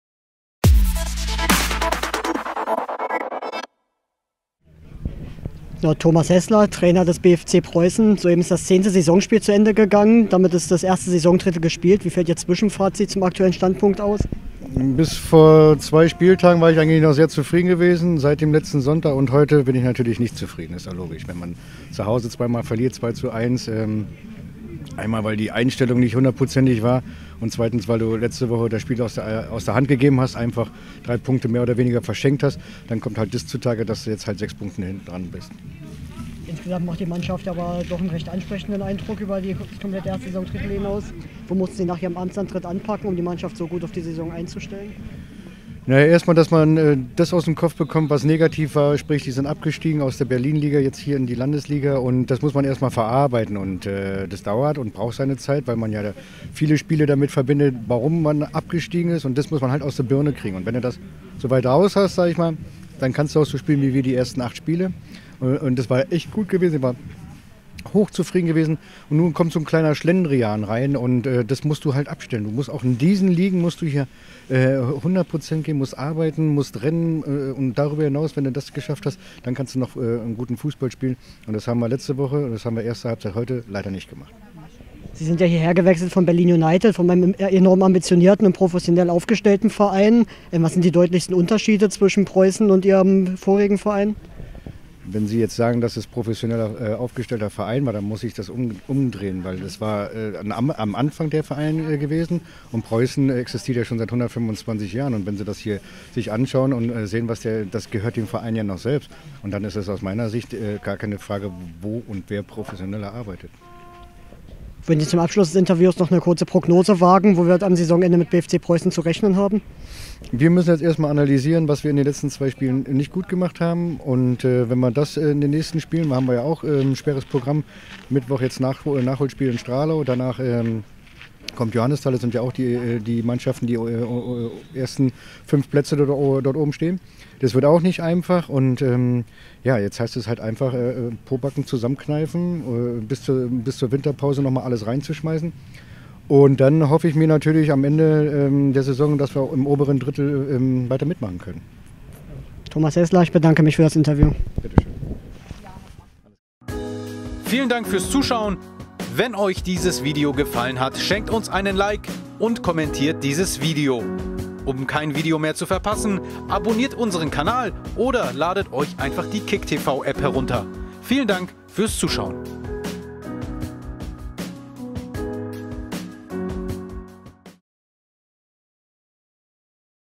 Der BFC Preussen im Aufstiegsrennen! – Thomas Häßler zieht die Zwischenbilanz
Thomas Häßler wechselte im vergangenen Sommer von Berlin United an die Seitenliene des BFC Preussen. Mit dem Landesligisten strebt er den direkten Wiederaufstieg an.